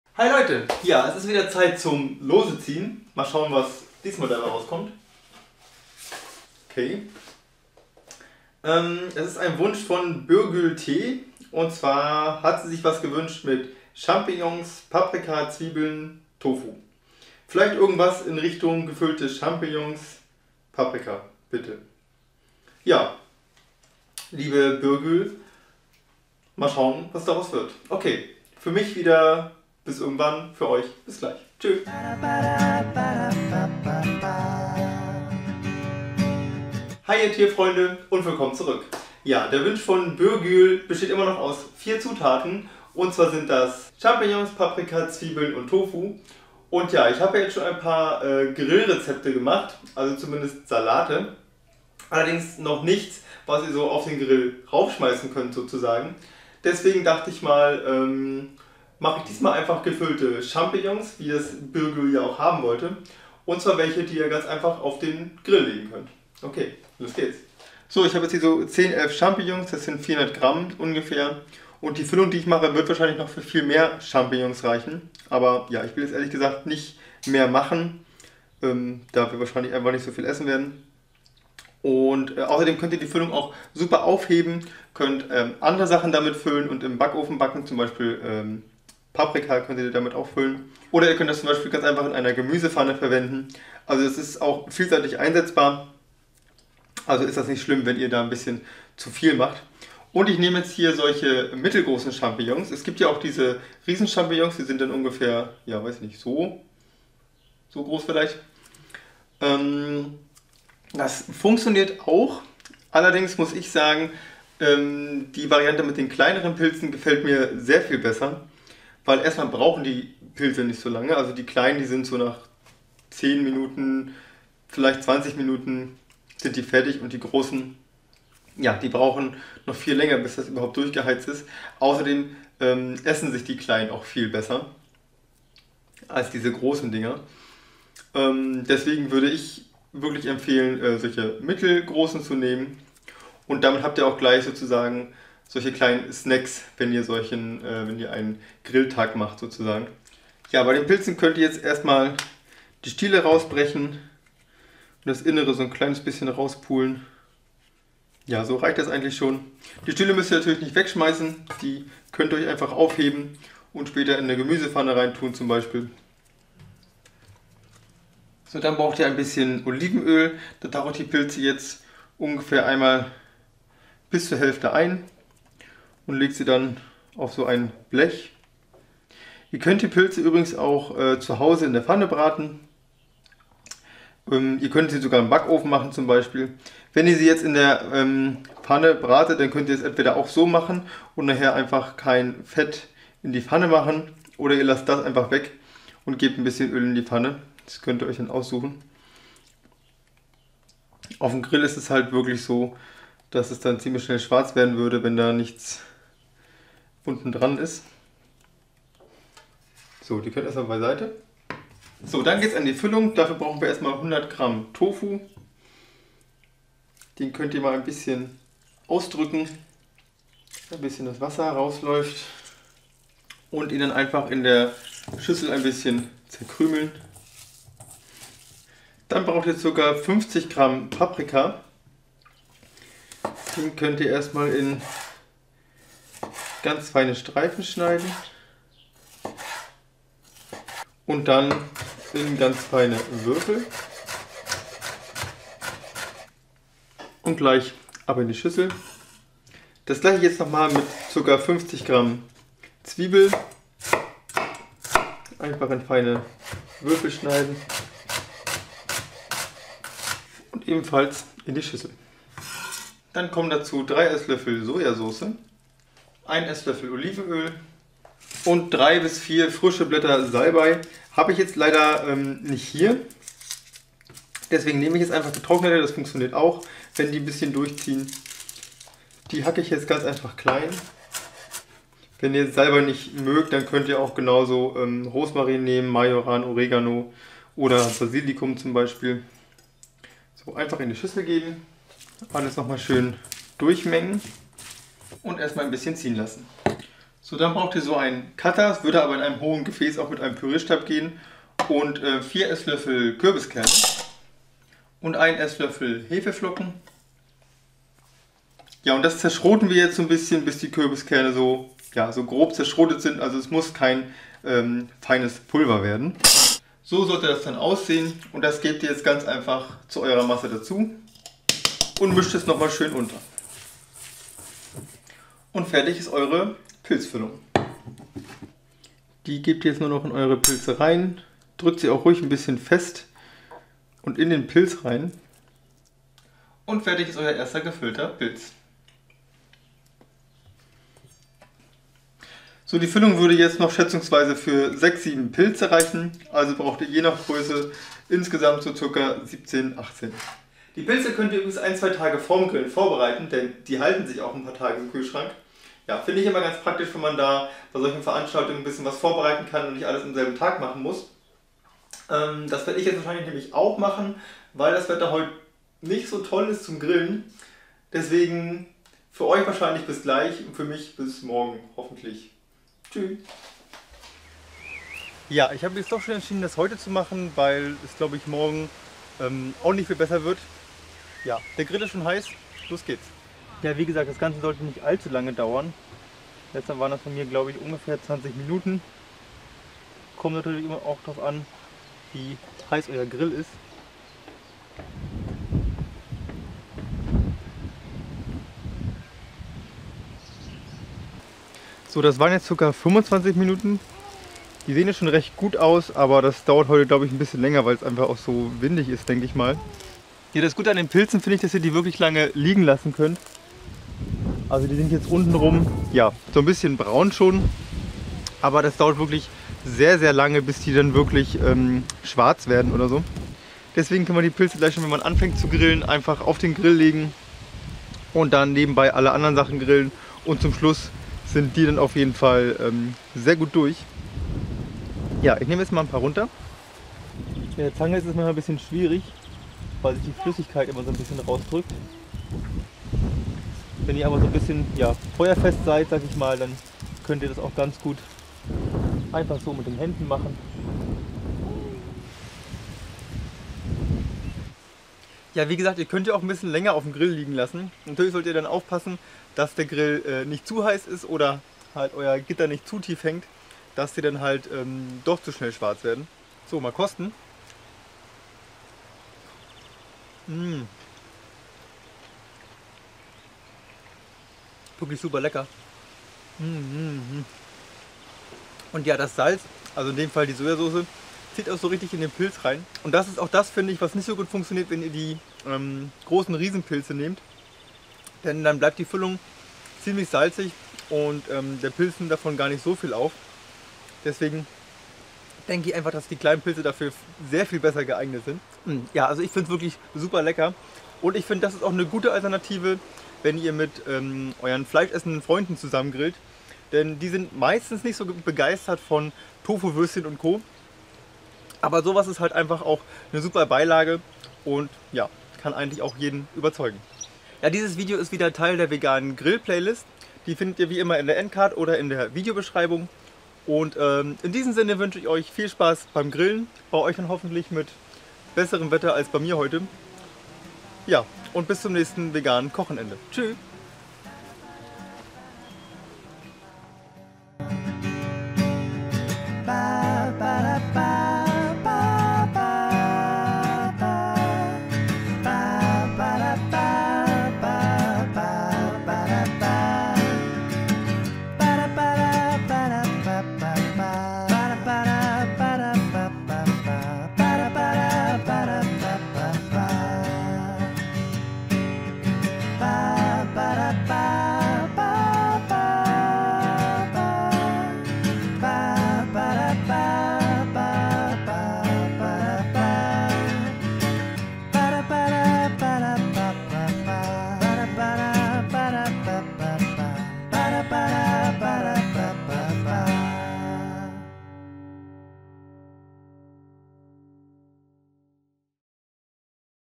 Gefüllte gegrillte Champignons - gegrilltes Gemüse - vegan grillen - vegane Rezepte von Koch's vegan
Endlich mal wieder ein Grillvideo. :) Zwar hat das Wetter nicht ganz so mitgespielt aber davon lassen sich echte Grillfans ja auch nicht abbringen^^. In diesem Video zeige ich euch wie ihr superleckere gegrillte gefüllte Champignons machen könnt. Eine super Alternative zu Tofuwurst und co. Schaut auch mal bei diesem Video vorbei Dort gibt's noch mehr gefülltes gegrilltes Gemüse. Also viel Spaß beim Grillen und bis zum nächsten veganen Kochenende.

Zutaten:
17 - 18 mittelgroße Pilze,
etwas Olivenöl,
100 g Tofu, 50 g Paprika,
1 Zwiebel (50 g),
3 EL Sojasauce, 1 EL Olivenöl,
3 - 4 fr. Salbeiblätter,
4 EL Kürbiskerne,
1 EL Hefeflocken